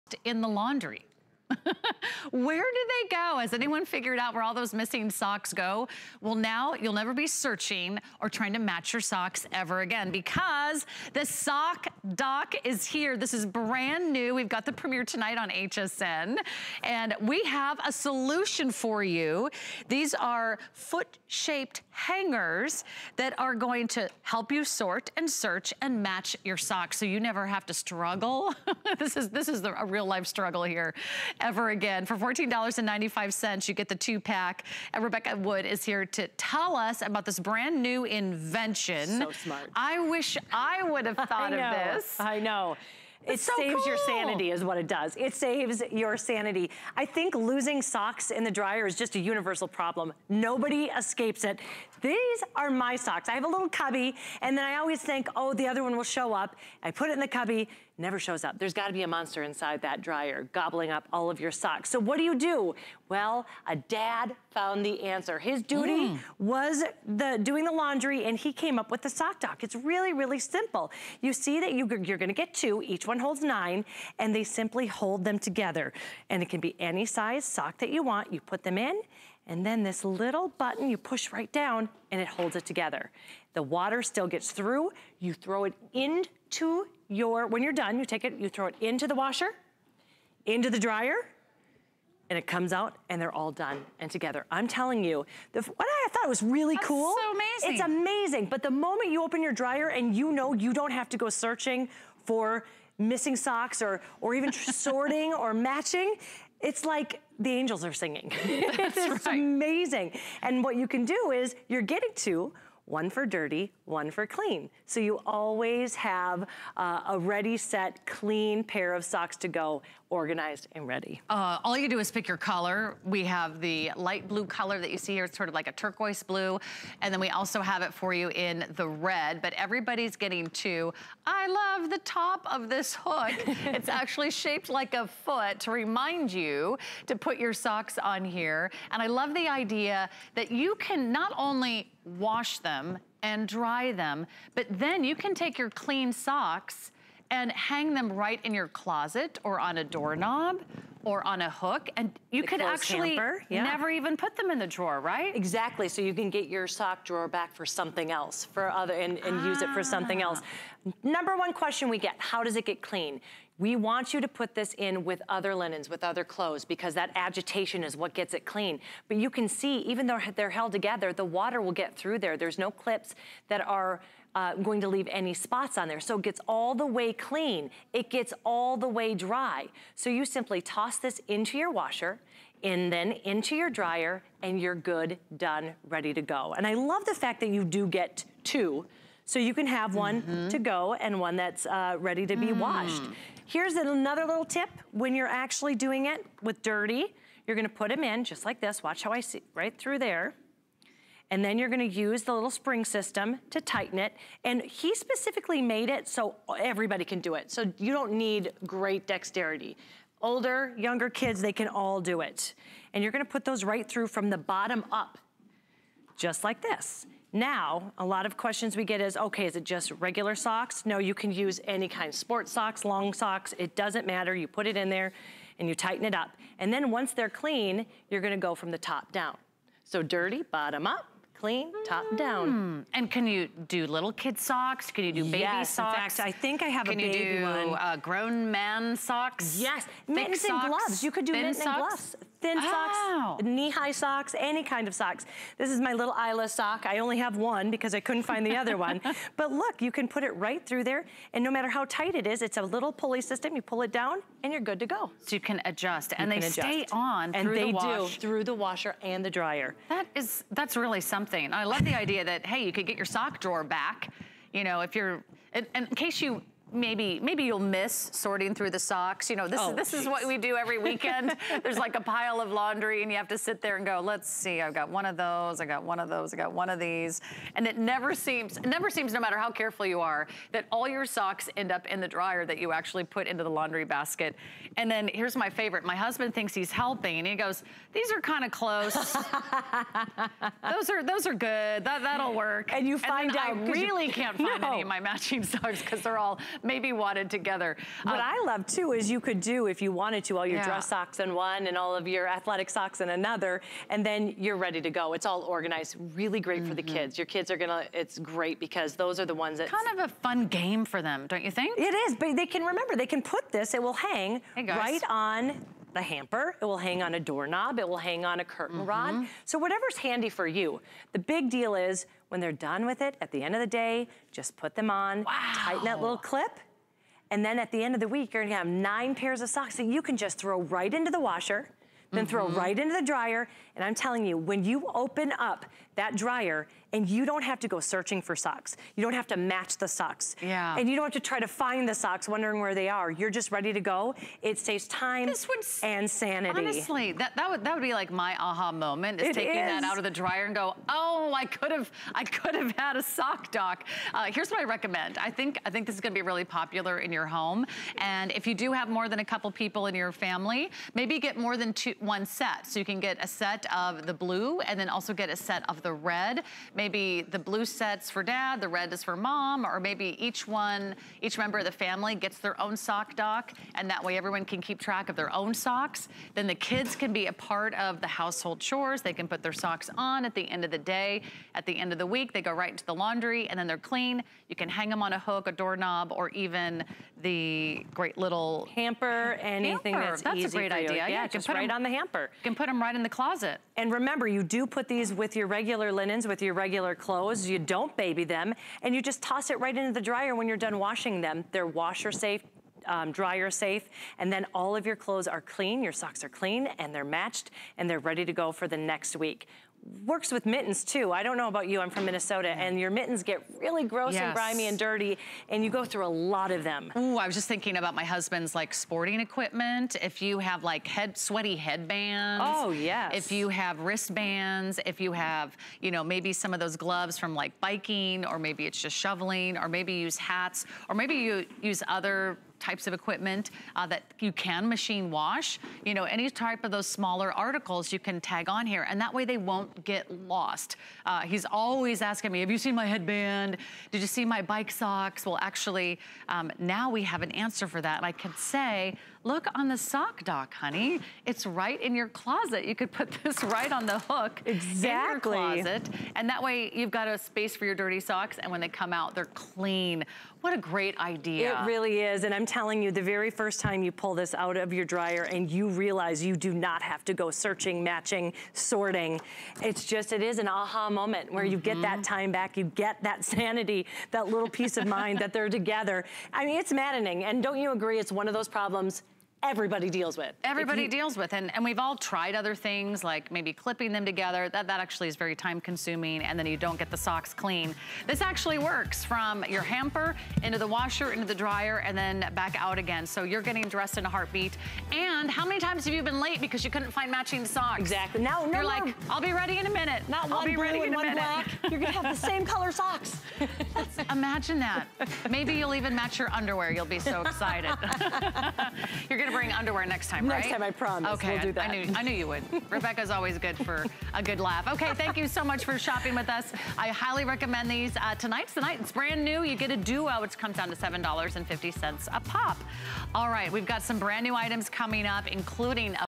SockDock Laundry Tool 2pack
Save your sanity and keep your socks sanitary with the SockDock. Socks remain in pairs while washing and drying, so you never have to sort, match or search for socks again. Clean socks are neatly organized by hanging in your closet, so you no longer need a sock drawer.

    2 Footshaped hangers

    2 Cords containing 10 adjustable buckles and 1 end clip